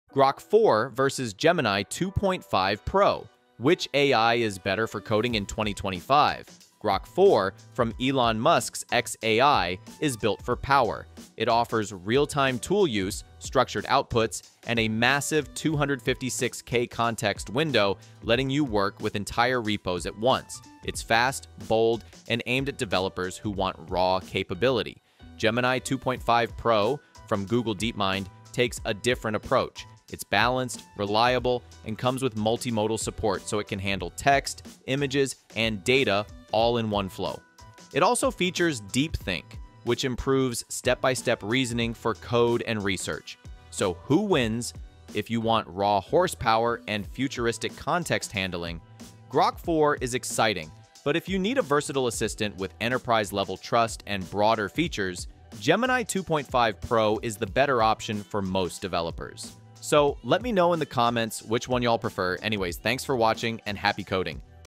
Can Grok 4 Beat Gemini 2.5 Pro in 2025?
Comparing Grok 4 and Gemini 2.5 Pro, both cutting-edge tools in the realm of artificial intelligence, helps developers make key decisions. This comparison highlights their strengths in coding with ai and llm capabilities for software engineering. Which of these ai tools is the better choice in the Grok vs Gemini showdown?